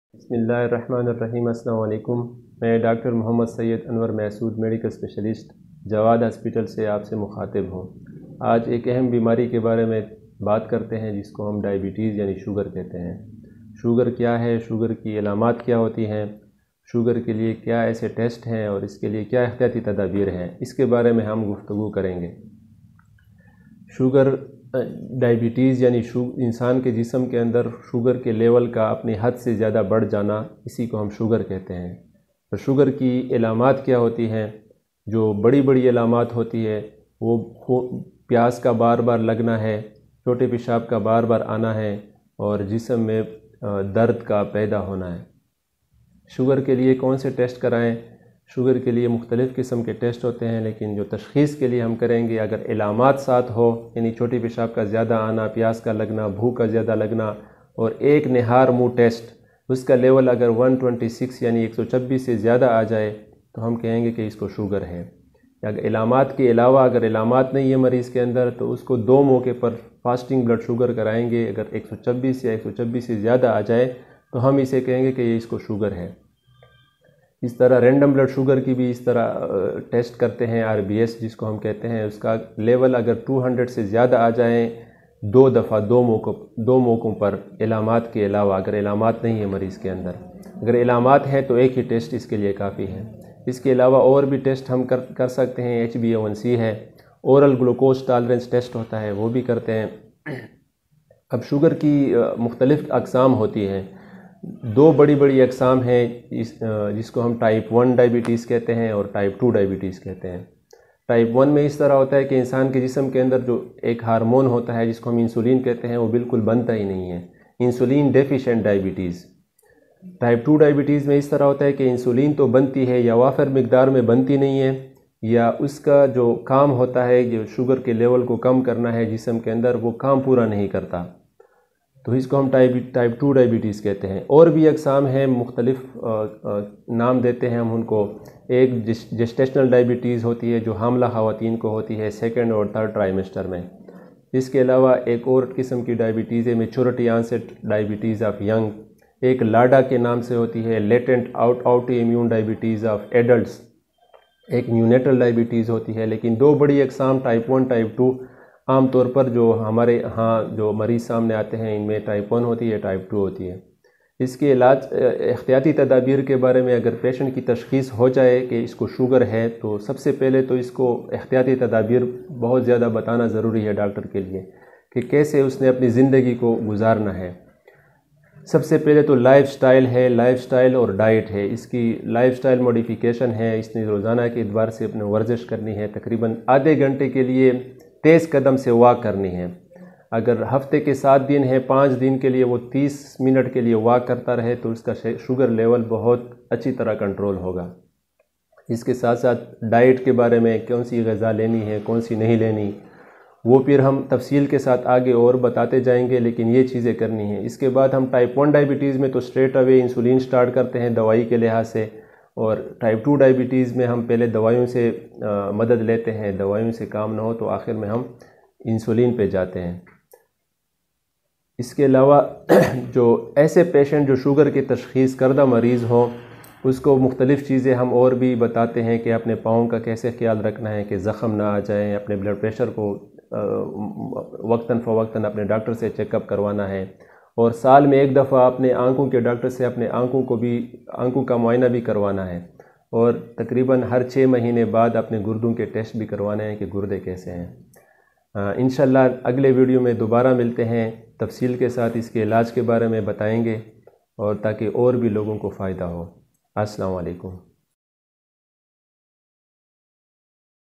Diabetes Melitus, Causes, Symptoms,Tests, Precautions and Treatment in Urdu/ Hindi
in this video Dr Muhammad Anwer Mehsood Medical Spacialist and Gold Medalist at Jawad Hospital Dera Ismail Khan information about Diabetes Melitus, Causes, Symptoms,Tests, Precautions and Treatment  in Urdu/ Hindi.